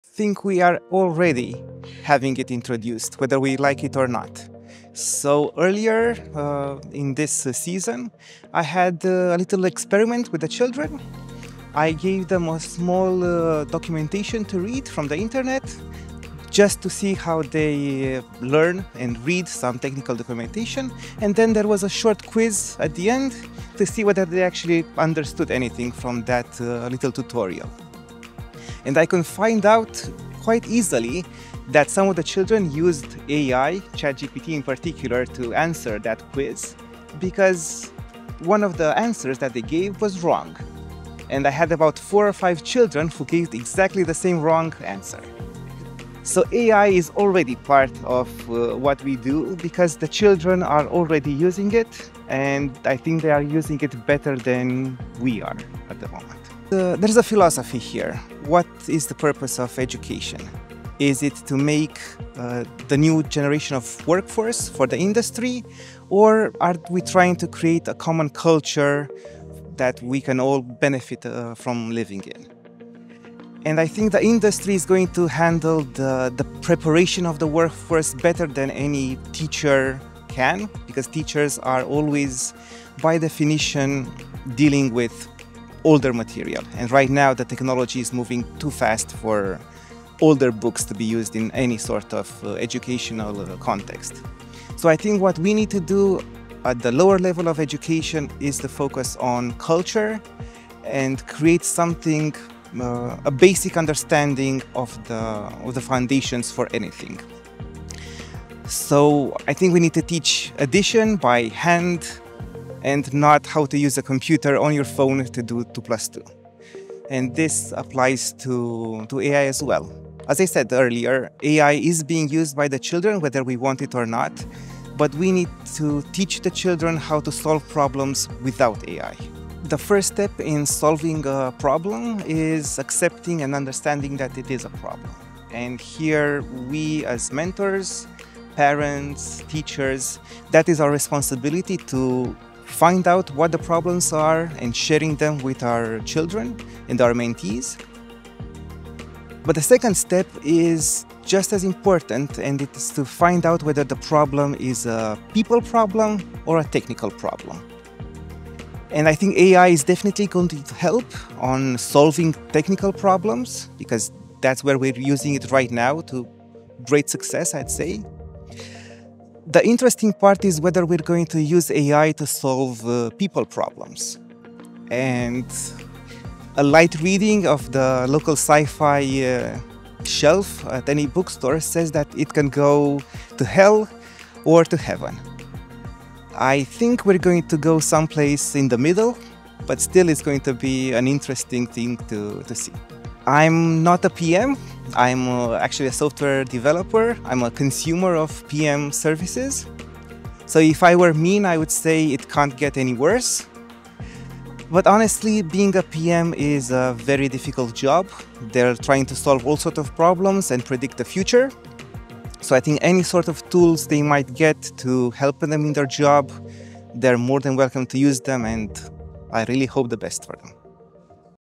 Interview with Radu Tebeica
PMI Romania - Annual Conference, Sep 27th, 2024
Navigating Tomorrow: Project Management for People, in the Age of Artificial Intelligence